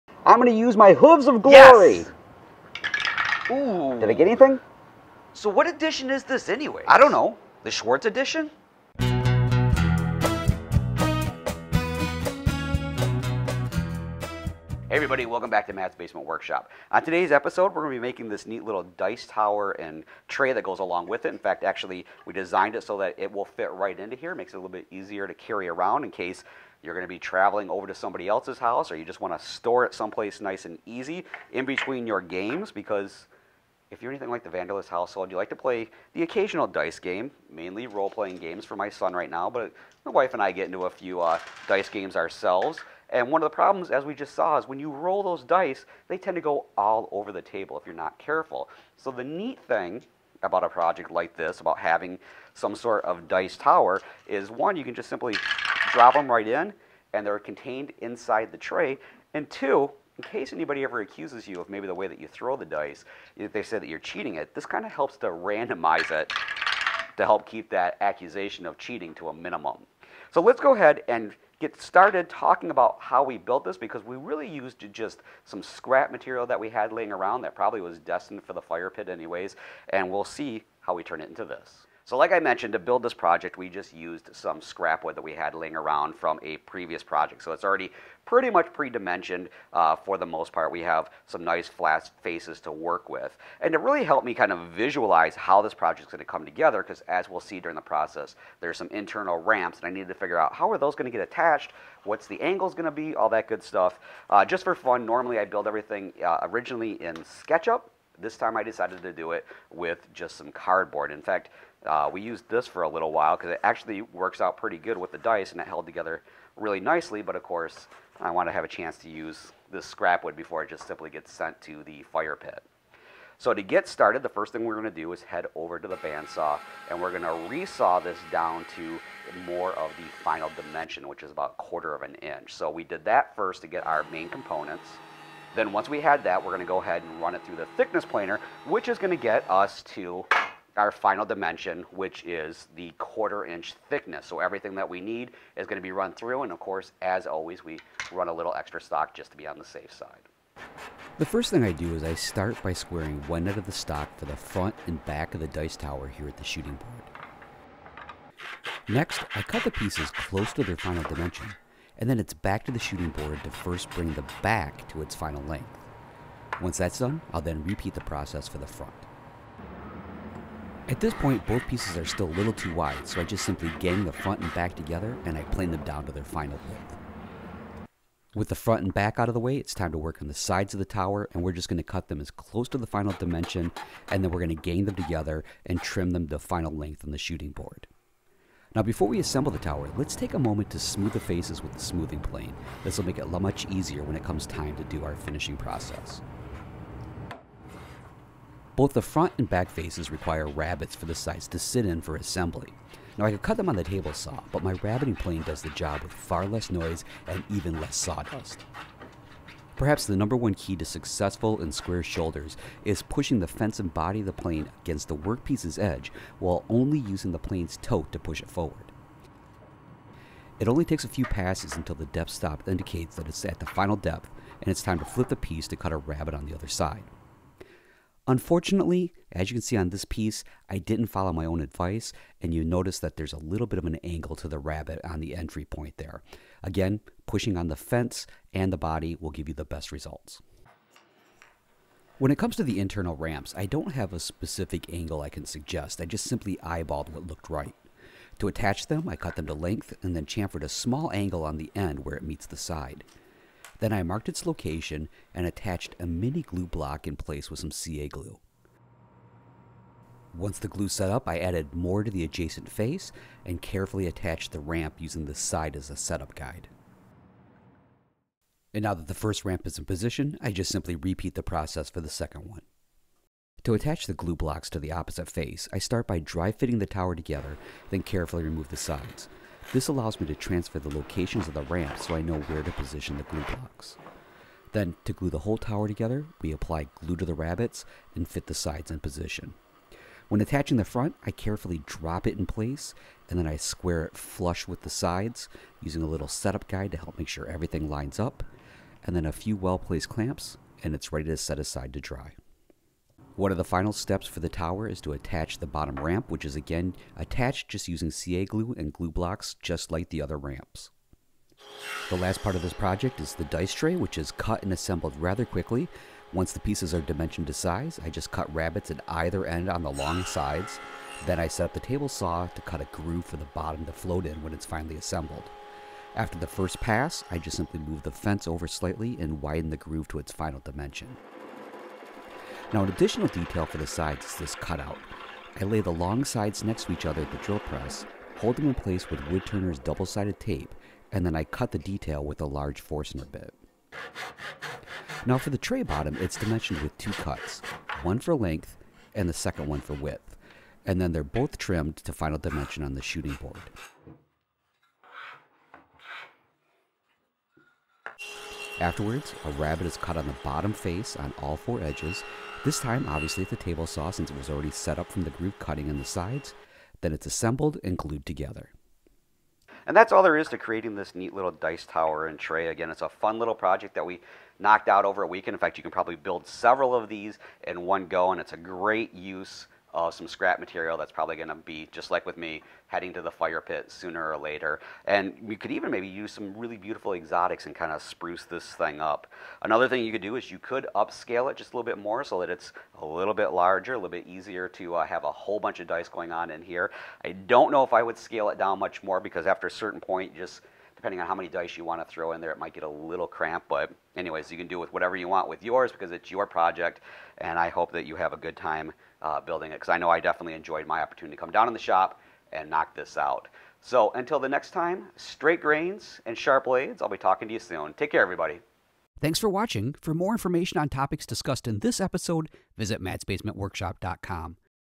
559 Dice Tower and Tray
Anyone who plays games involving dice there’s two things you always encounter; 1) they inevitably go flying off the tabletop, or 2) someone claims you threw them in a way that meant you were cheating. Aside from building a gaming table with raised sides like a craps table in Vegas your options can be limited on how to fix the problem.

A quick little project I built to help keep the dice in play is a "Dice Tower and Tray.” I made mine out of scrap poplar taking up space in the lumber rack and it only took a couple of hours on a Saturday afternoon.